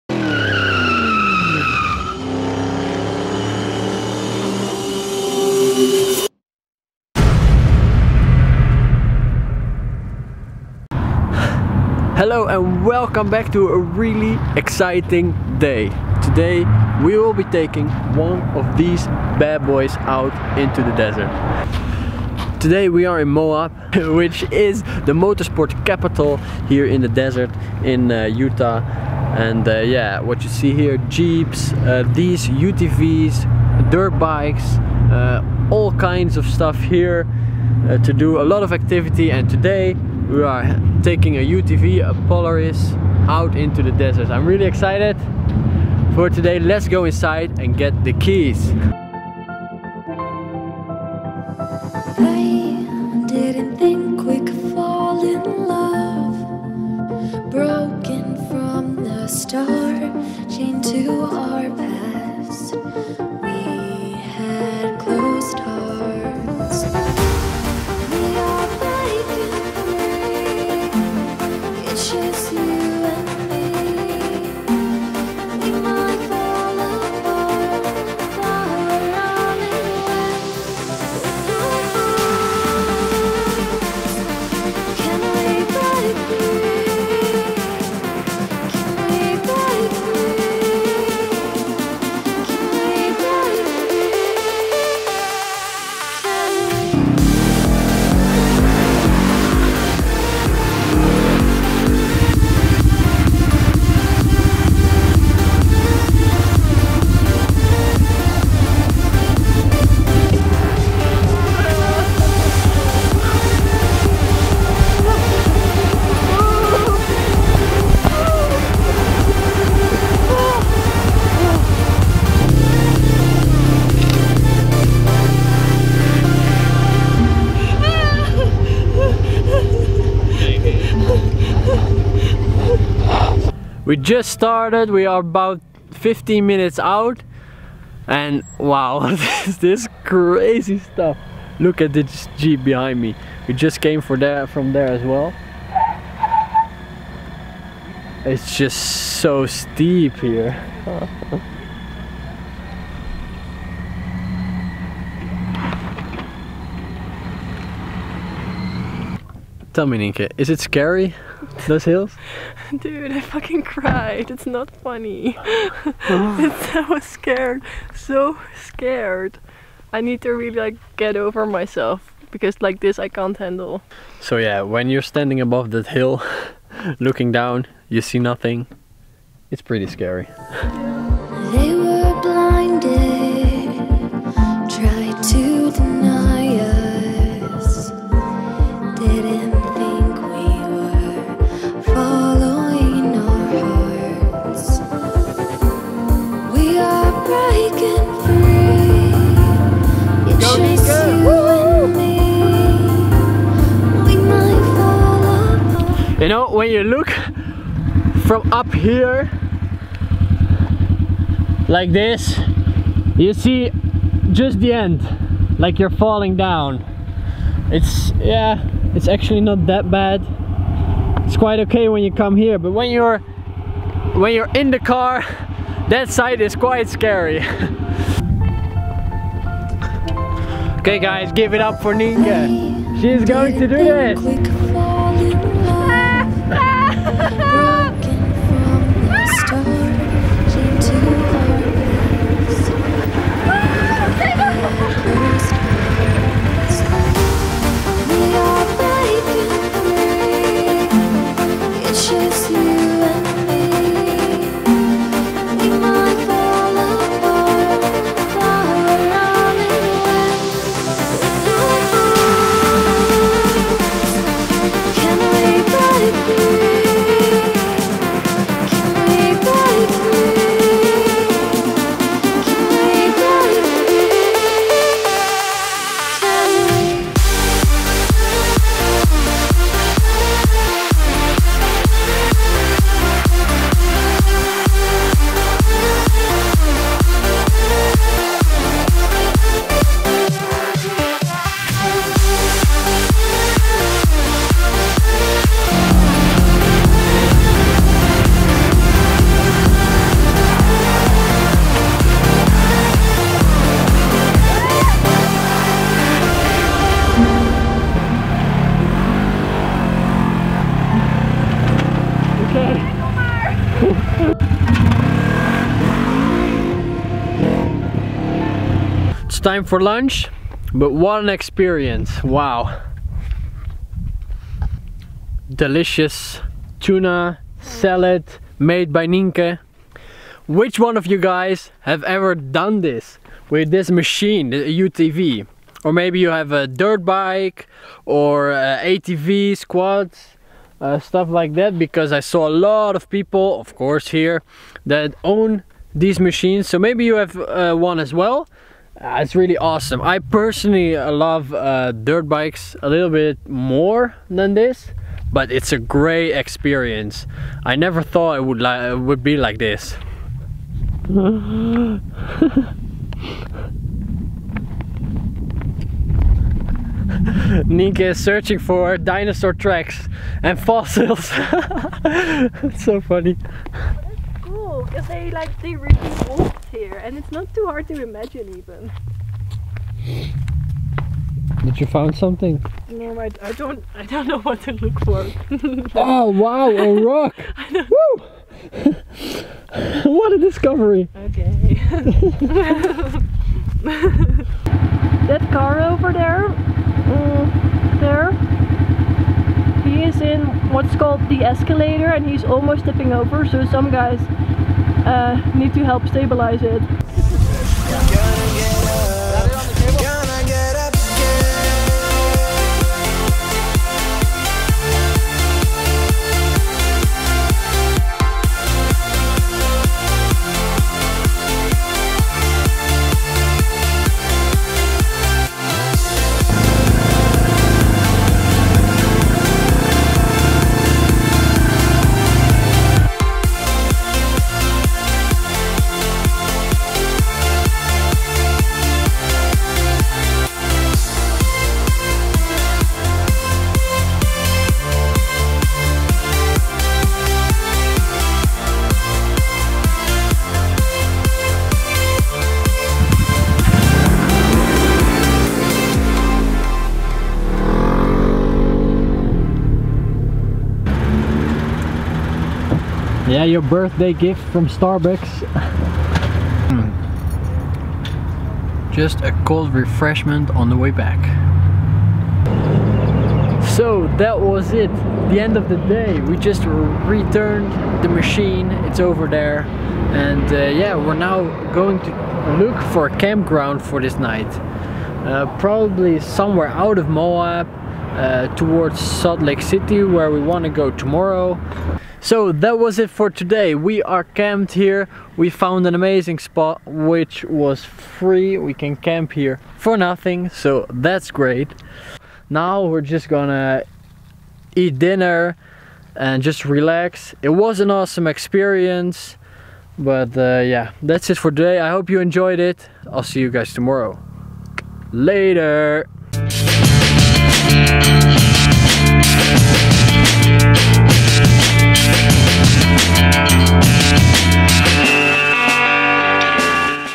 She crashed her car!! Moab | VLOG #21
Today we really had a blast going off road with a UTV. If you are in Moab you cannot escape the motorsport. We just had to join. We rented a UTV and got out of Moab to the off road trails. We did Fish n Things and Hells revenge! Awesome day, I hope you enjoy!